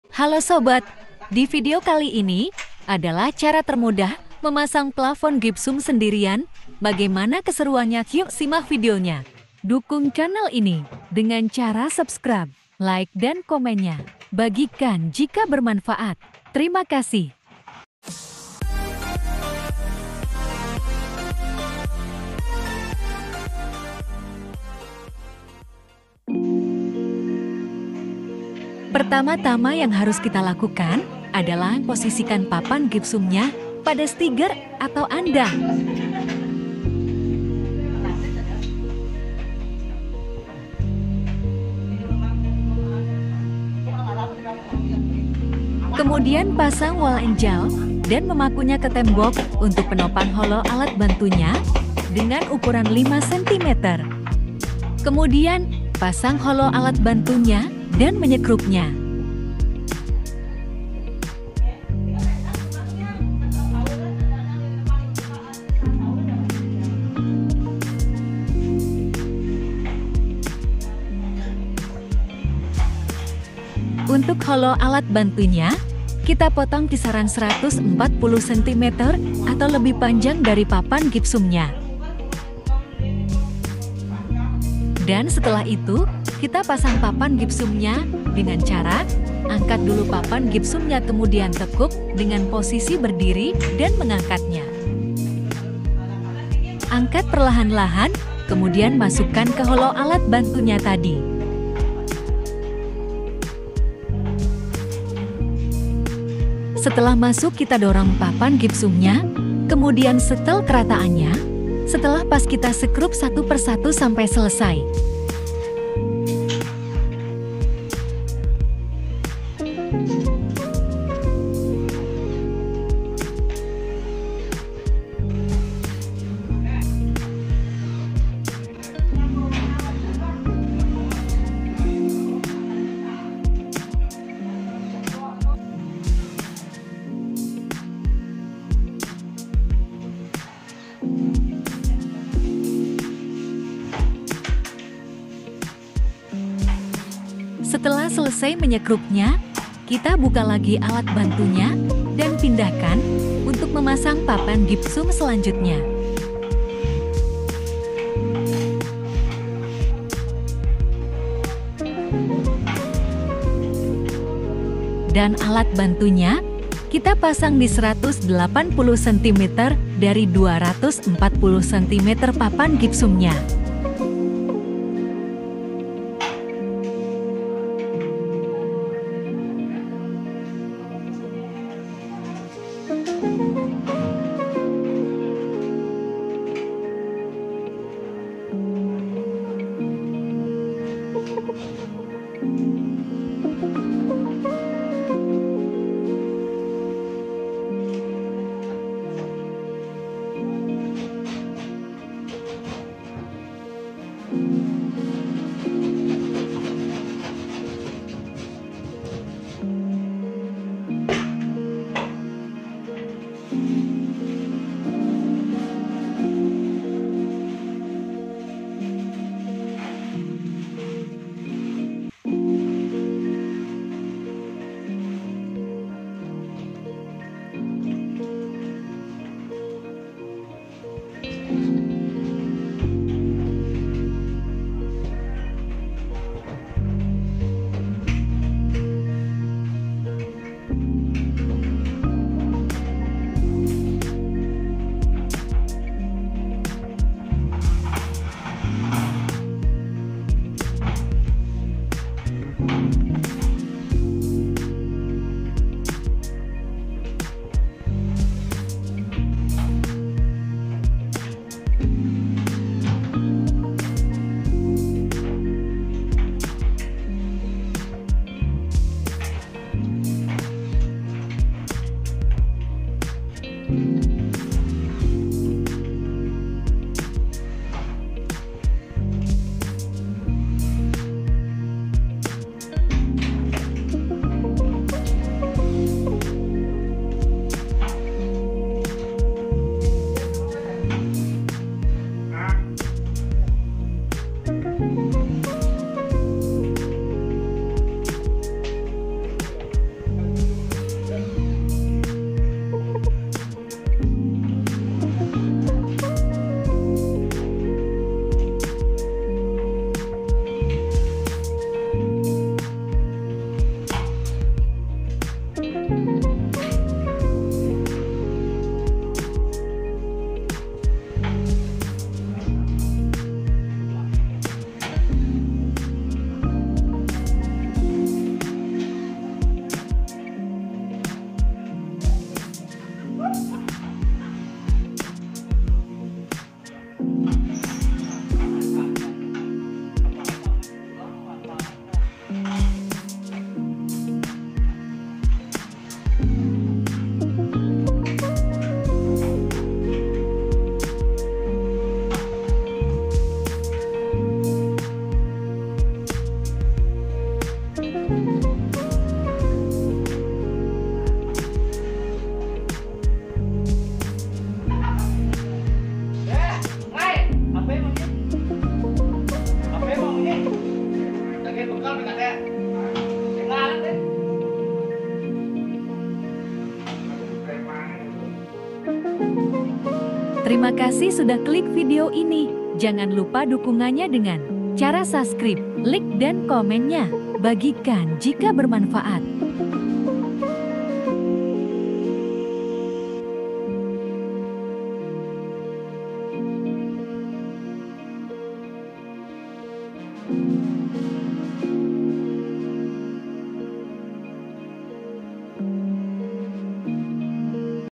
Cara pasang papan gypsum sendirian 2,4×1,2 dengan utuh bagi pemula
Pasang papan gypsum sendirian 2,4×1,2 dengan utuh
Sangat mudah sekali bagi para pemula yg mau pasang plafon gypsum sendirian.
Hanya dengan alat bantu hollow ukuran 1,4 atau lebih panjang dari papan gypsumnya dan sekrup ulir ukuran 5cm. Kita pasang alat bantunya di hollow 60 cmnya. Dengan membuat space 1,5 cm supaya papan gypsumnya bisa masuk.